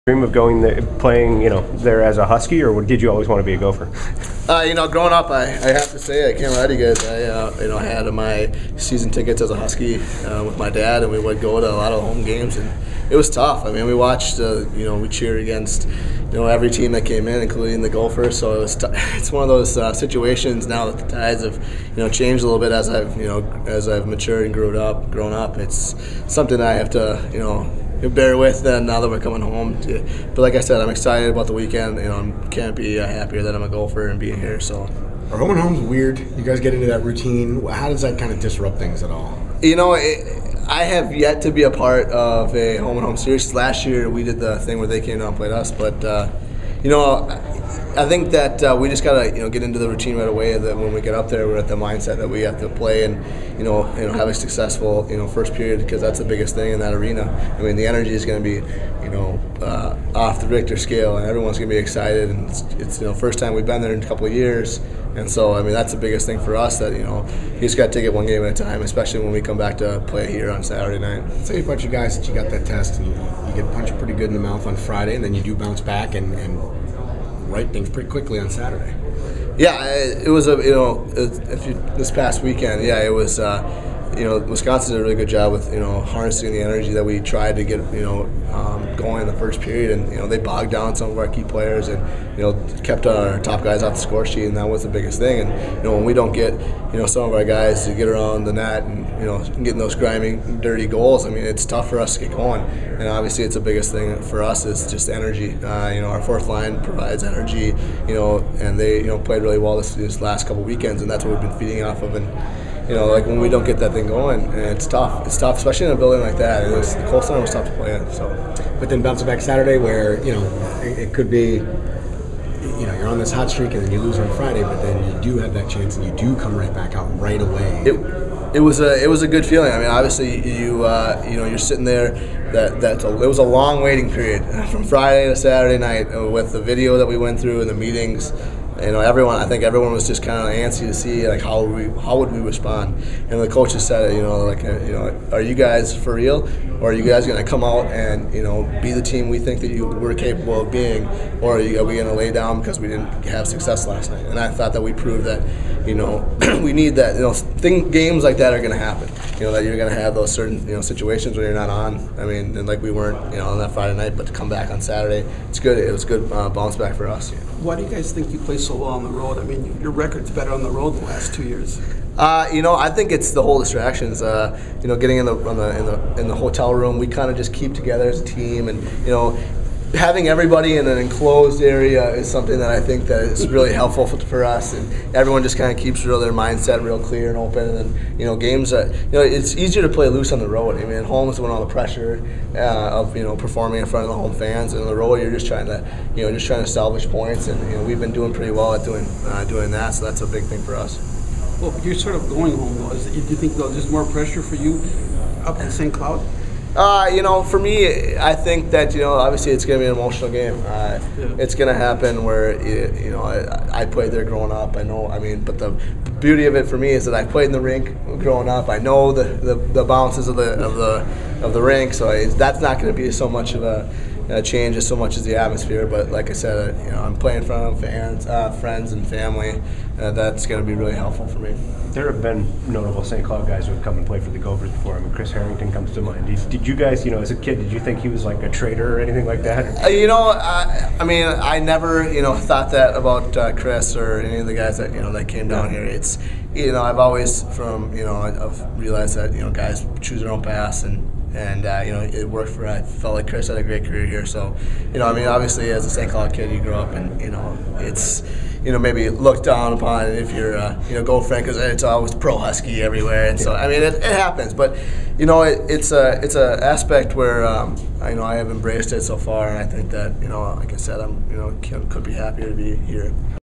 Minnesota defenseman Nate Schmidt - Nov. 16, 2011
Gophers defenseman Nate Schmidt talks about going home to St. Cloud to face the Huskies.